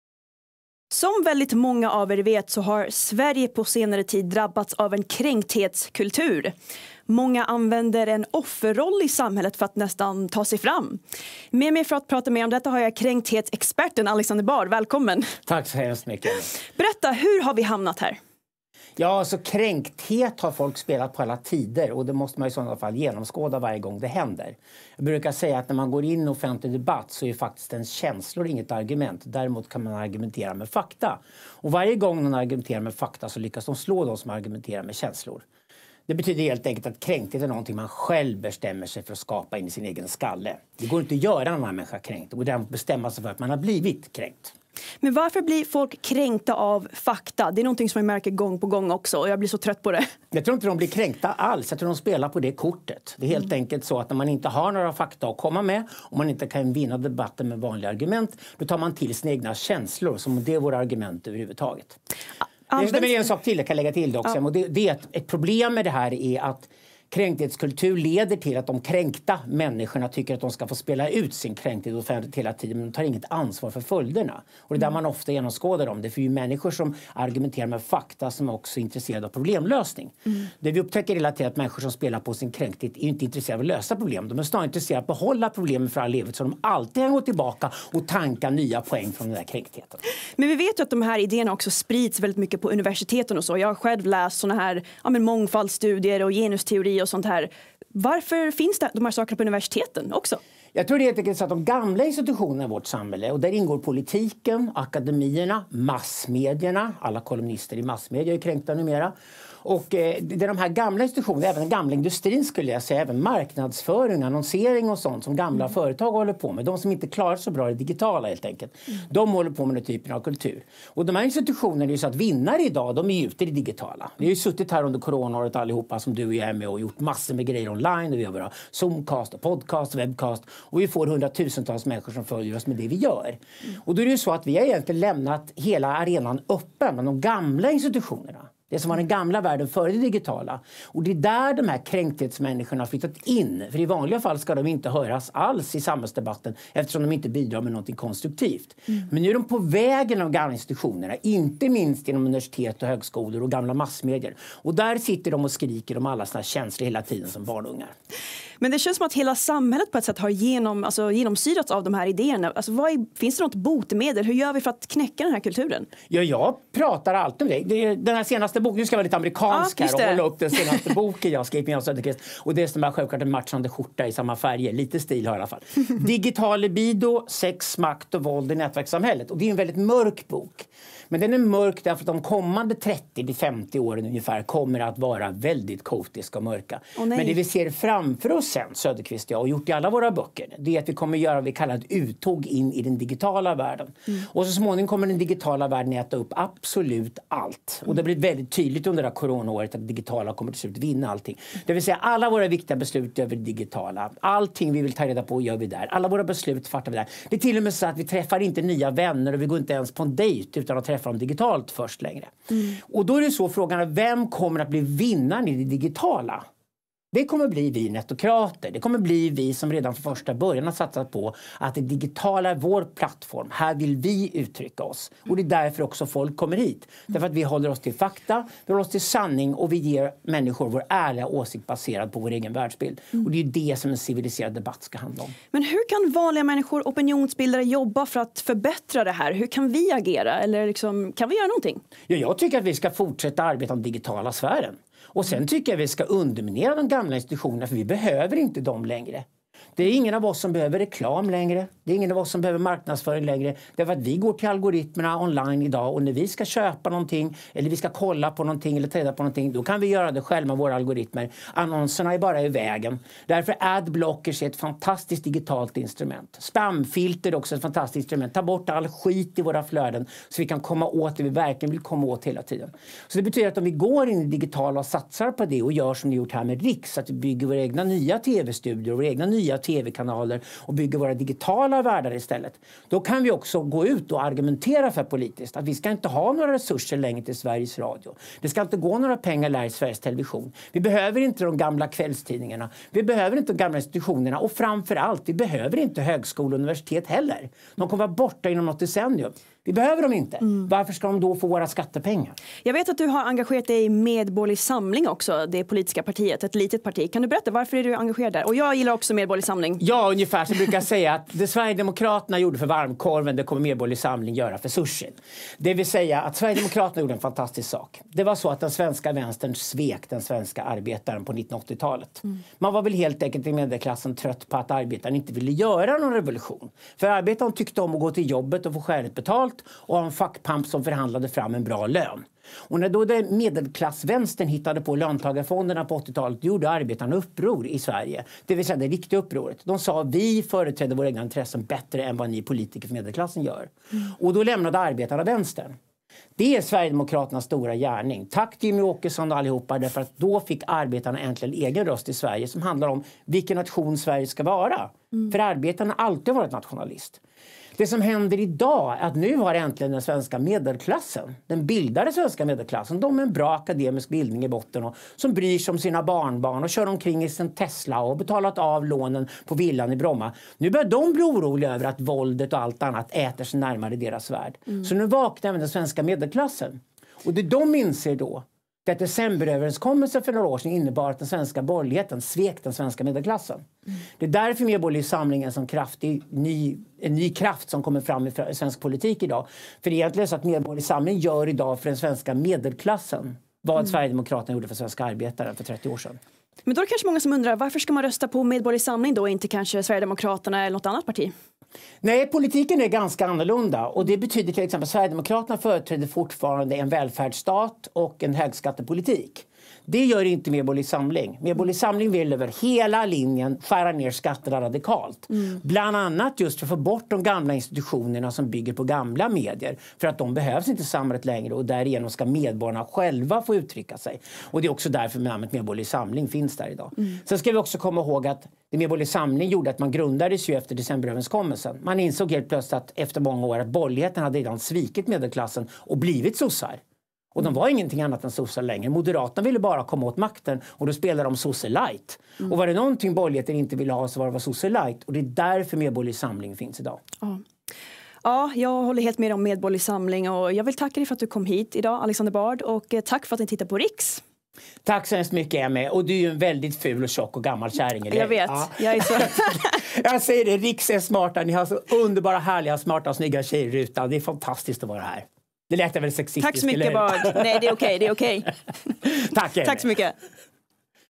Alexander Bard - känslor är inget argument
Emmie intervjuar Alexander Bard, om kränkthet, om hans nya bok och lite allt möjligt.